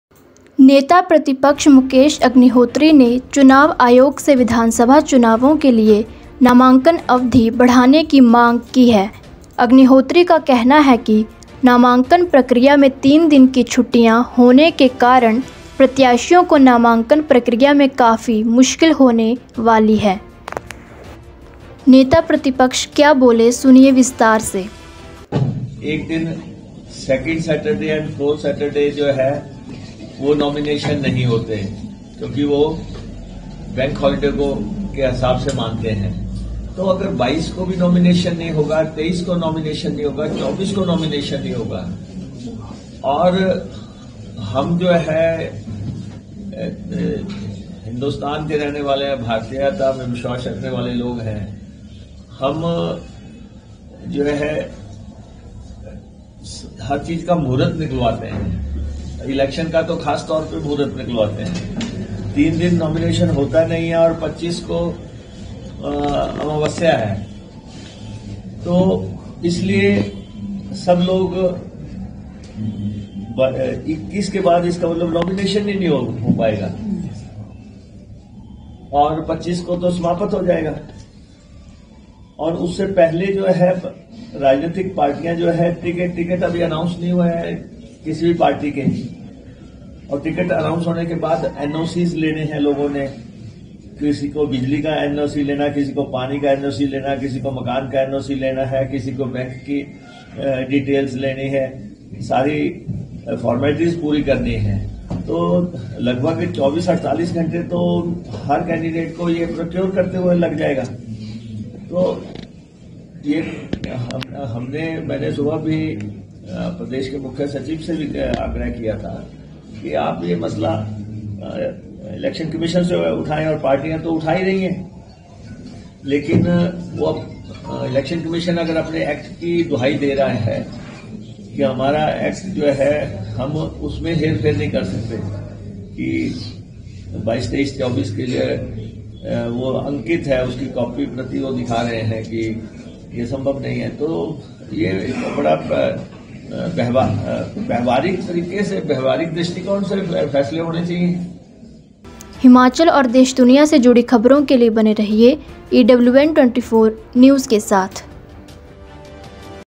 विधानसभा चुनावों के लिए नामांकन अवधि बढ़ाई जाए, ऐसा क्यों बोले अग्निहोत्री सुनिए | Himachal
Leader of Opposition Mukesh Agnihotri has demanded the Election Commission to extend the nomination period for the assembly elections. Agnihotri says that the candidates are going to face a lot of difficulty in the nomination process due to the holidays being 3 days in the nomination process. Listen in detail to what the Leader of the Opposition said...